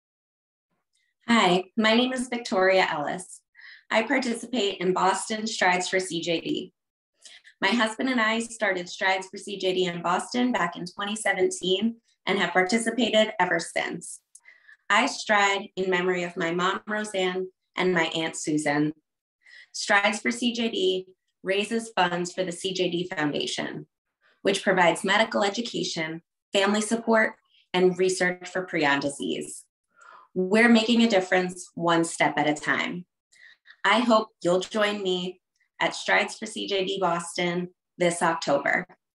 Strides for CJD Boston 2022
Meet Victoria Costello the Boston Strides for CJD founding captain.